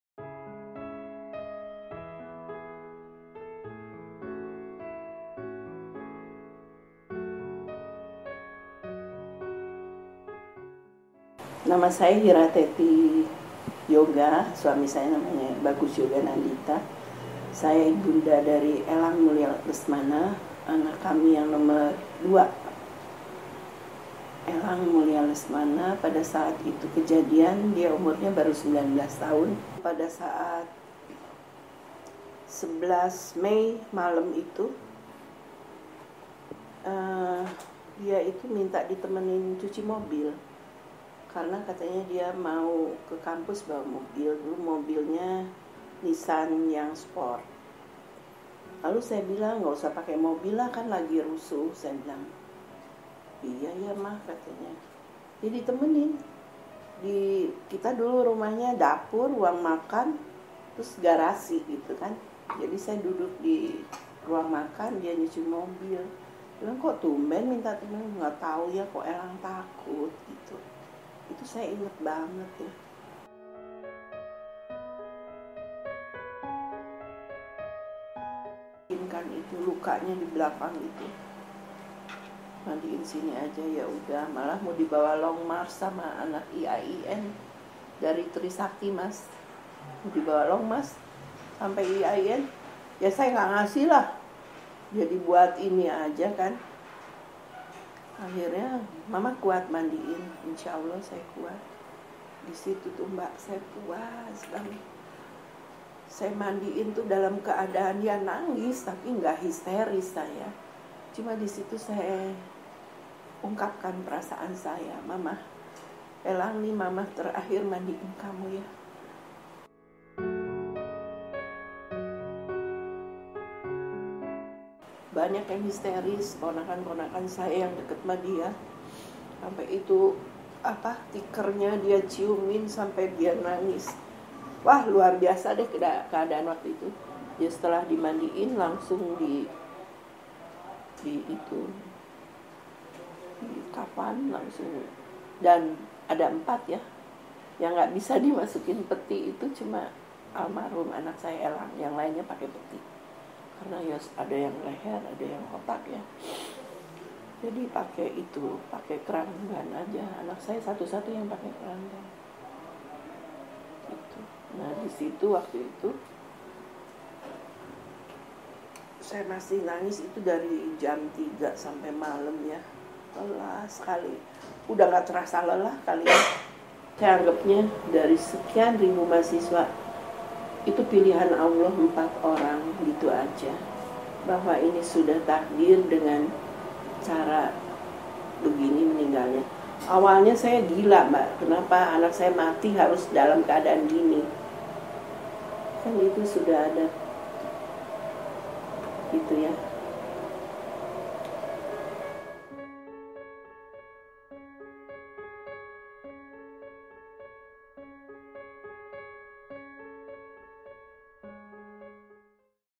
MenolakLupa - Hira Tety Yoga (Ibu dari Elang Lesmana, korban penembakan tragedi Trisakti 1998)
Ibu Hira Tety Yoga adalah ibu kandung dari korban penembakan oleh aparat didalam halaman Universitas Trisakti pada tahun 1998.